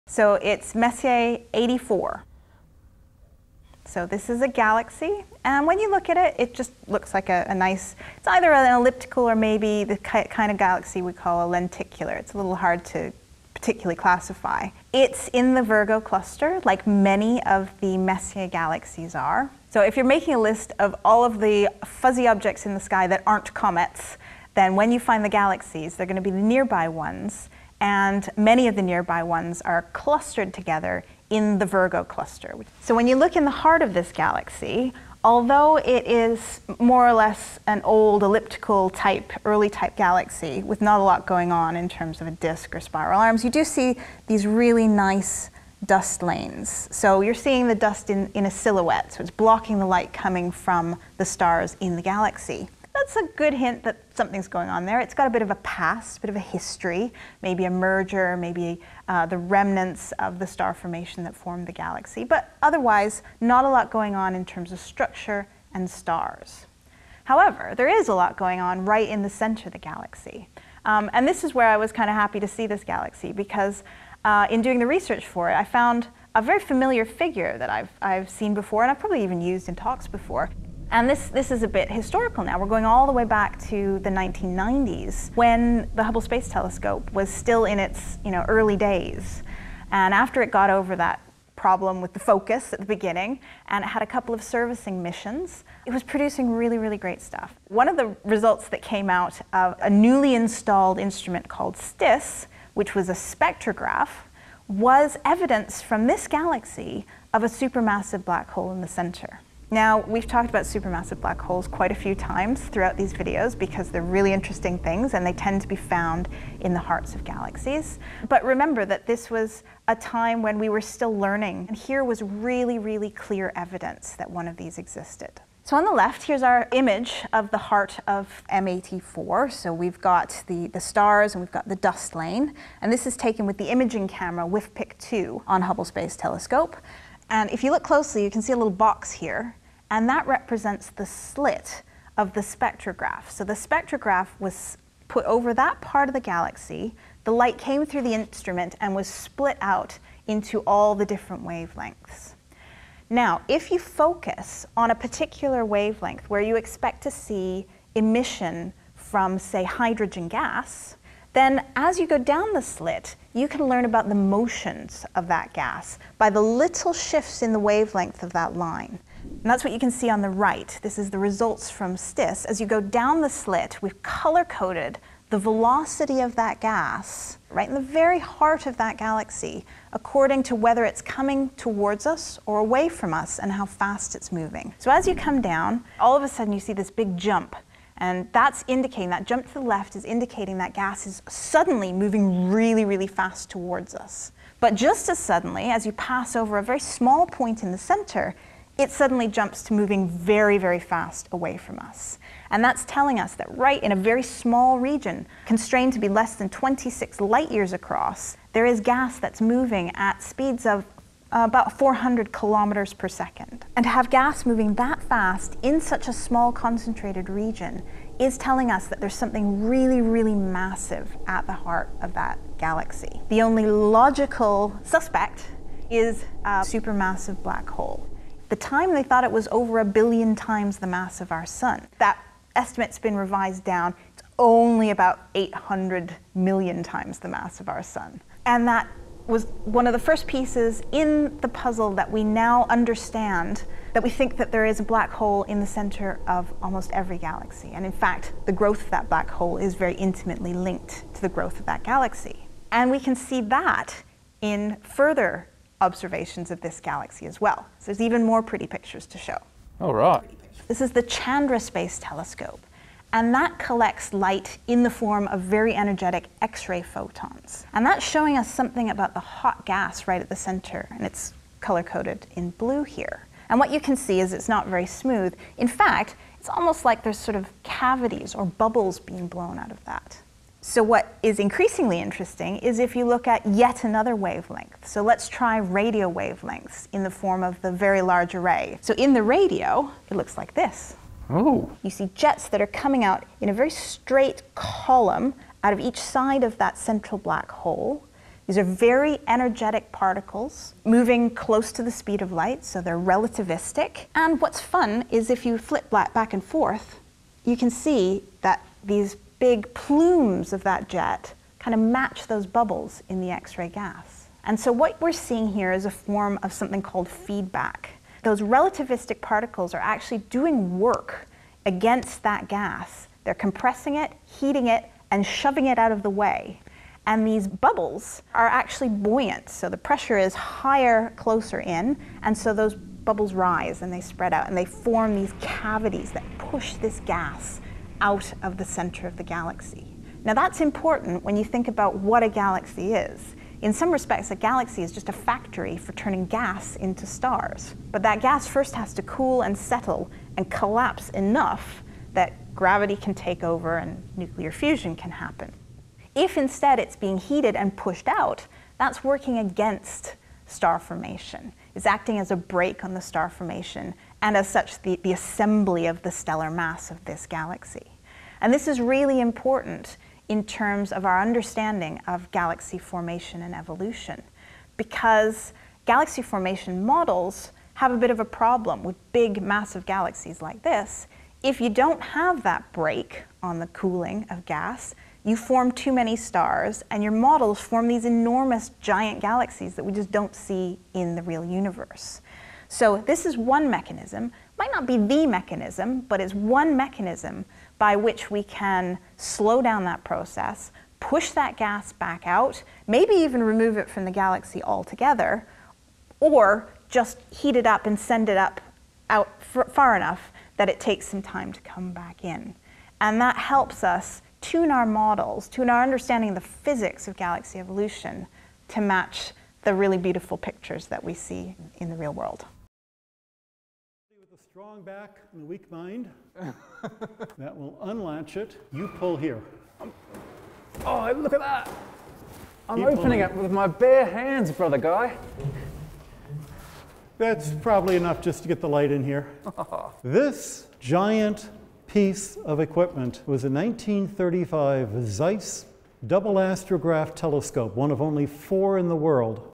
M84 - Feedback and Bubbles - Deep Sky Videos
Dr Meghan Gray discusses Messier 84 - a Lenticular Galaxy with a turbulent centre.
More links and info in full description ↓↓↓

Dr Gray is an associate professor at the University of Nottingham

Video by Brady Haran and James Hennessy